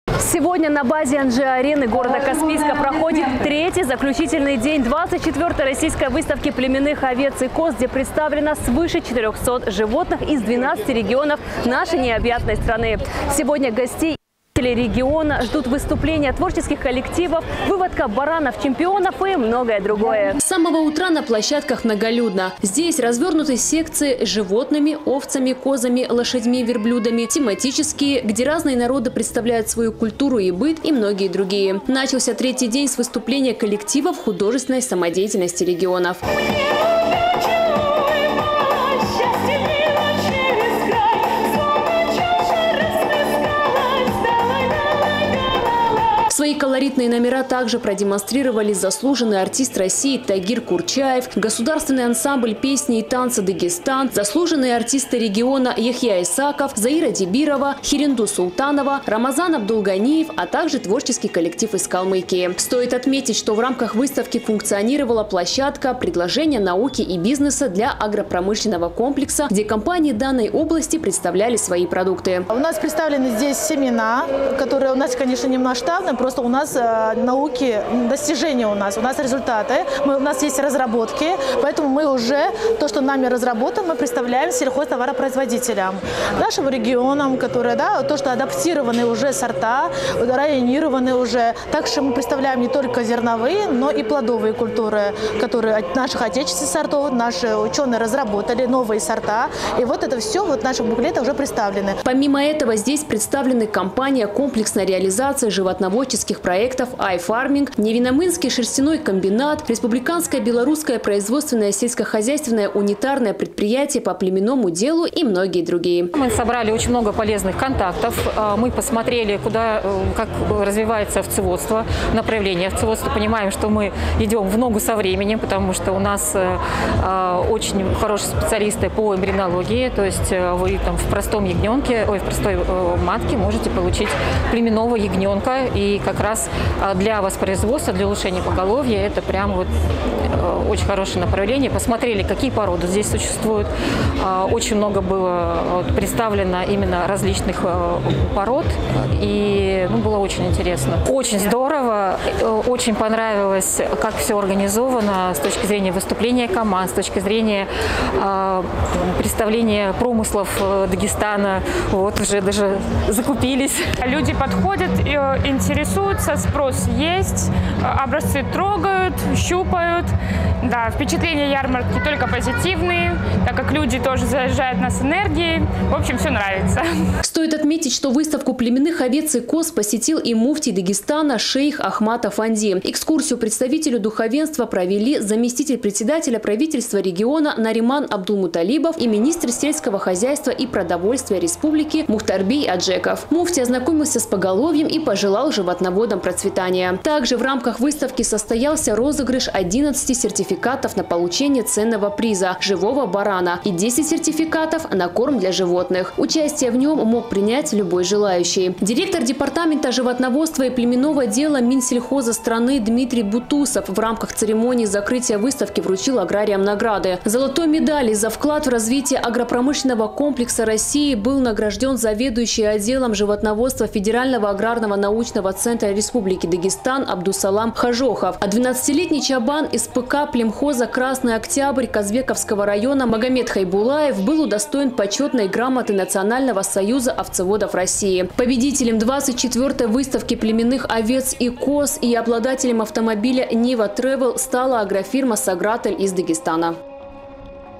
Победителем 24-й Всероссийской выставки племенных овец и коз стала дагестанская агрофирма «Согратль»
С самого утра на площадках многолюдно. Здесь развернуты секции с животными - овцами, козами, лошадьми, верблюдами; тематические, где разные народы представляли свою культуру и быт. Начался третий день с выступления коллективов художественной самодеятельности регионов.

Свои колоритные номера также продемонстрировали заслуженный артист России Тагир Курчаев, Государственный ансамбль песни и танца «Дагестан», заслуженные артисты Дагестана Яхья Исаков, Заира Дибирова, Хиринду Султанова, Рамазан Абдулганиев, а также творческий коллектив из Калмыкии.

Стоит отметить, что в рамках выставки функционировала площадка «Предложения науки и бизнеса для агропромышленного комплекса», где компании данной области представили свои продукты.